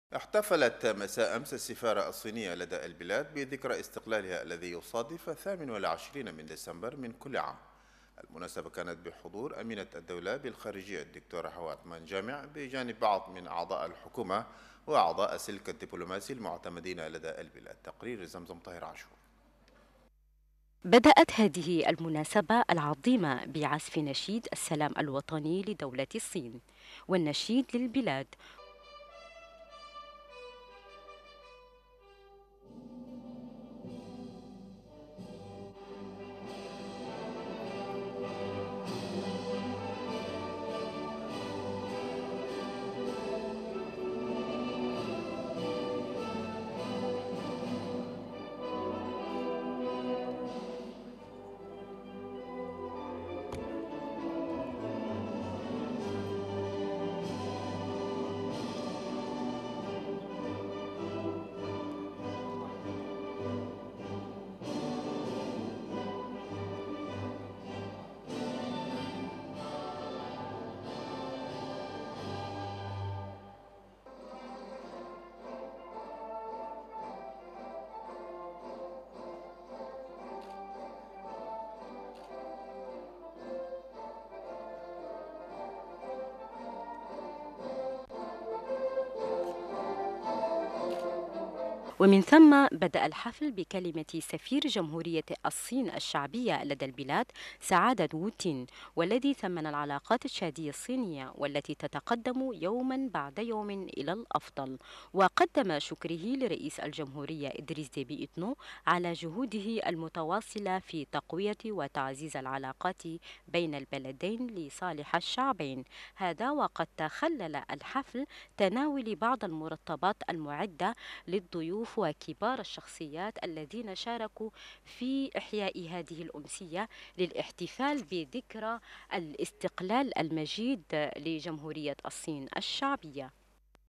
نشرة تلفزيون تشاد يوم 29 سبتمبر 2017 - تشاد / الصين
العنوان
1) السفارة الصينية إحتفلت بذكرى إستقلالها.